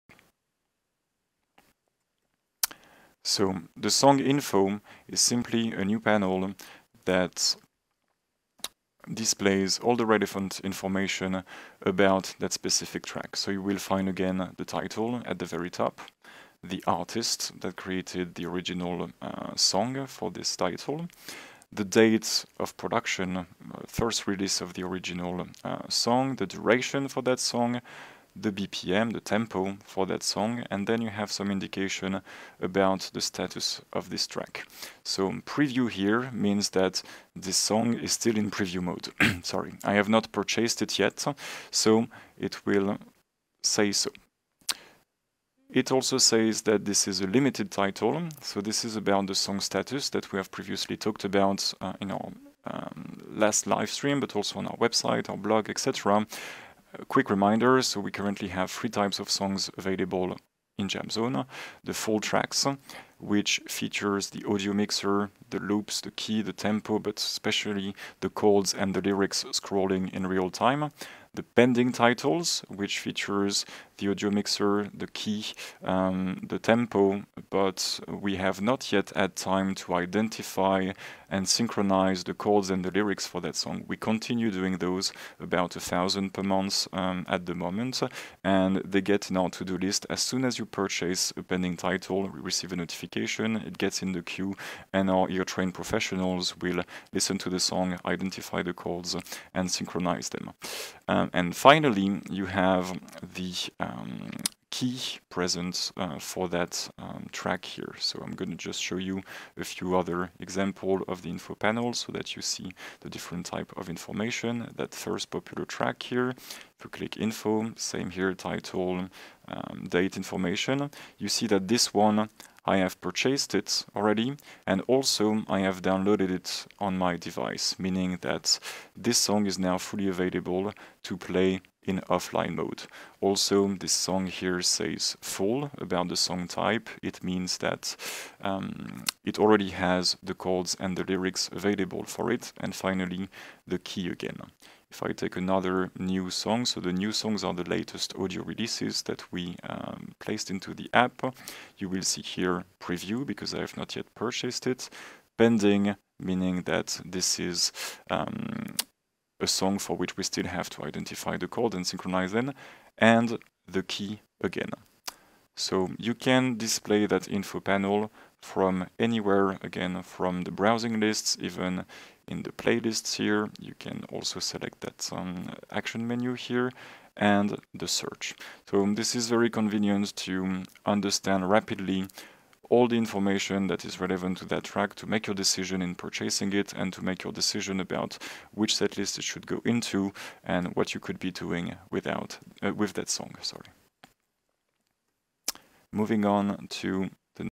Jamzone App: Song Info Panel (tutorial)
In this video we demo the new features available from Jamzone v2.18 on Apple and Jamzone v2.8 on Android.

How to display the song information relative to a track.
- Title
- Artist
- Release Year
- Duration
- BPM
- Download status
- Ownership status
- Song type (Full, Pending, Limited)
- Key(s)